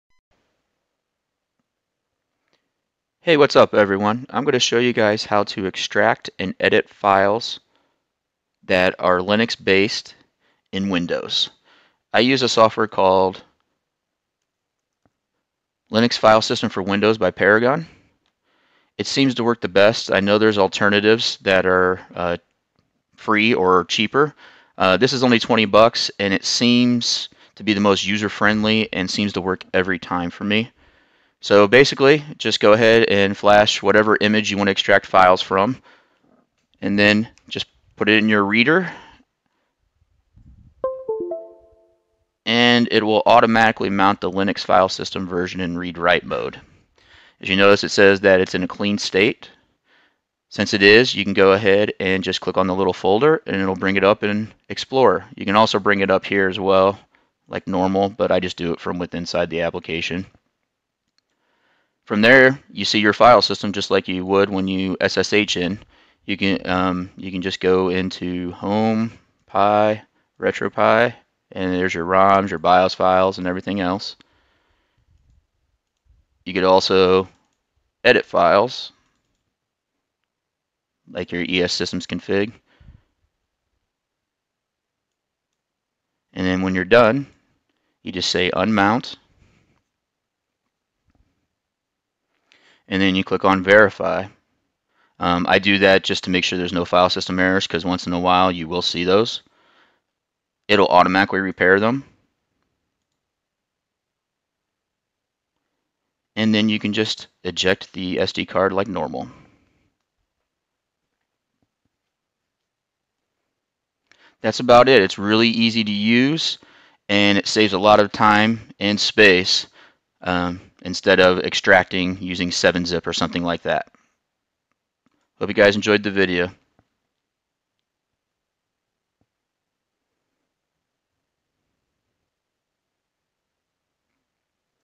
How to Extract and Edit Linux Files in Windows
This software makes extract and editing linux files in WIndows a snap. It saves a ton of time and makes things a little easier for people that are not fond of the command line